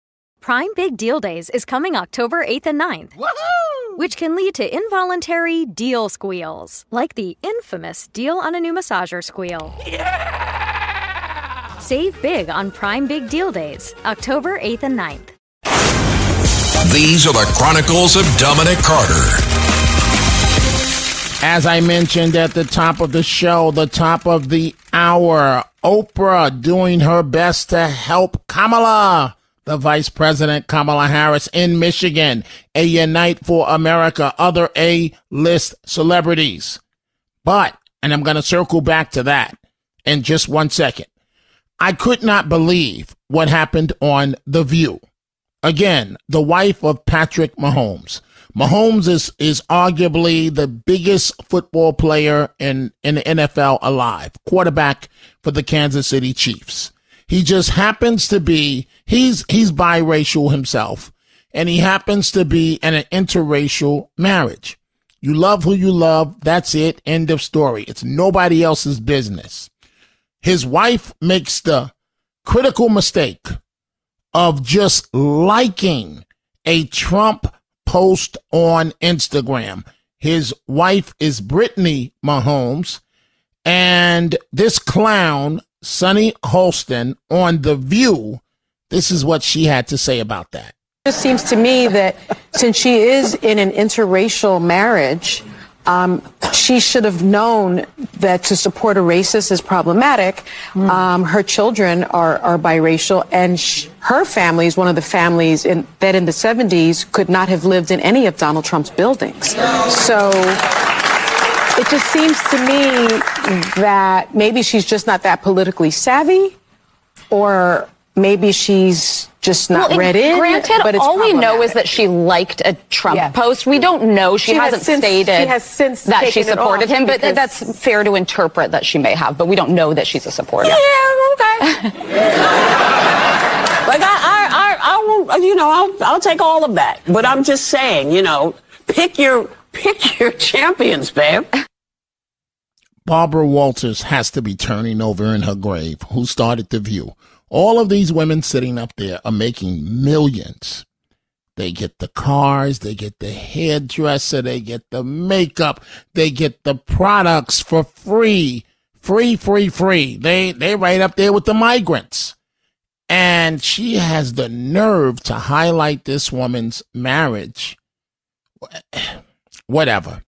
The Chronicles of Dominic Carter | 09-20-24
Dominic talks about scathing comments made on The View about Brittany Mahomes, NFL quarterback Patrick Mahomes' wife, simply liking an Instagram post from Donald Trump.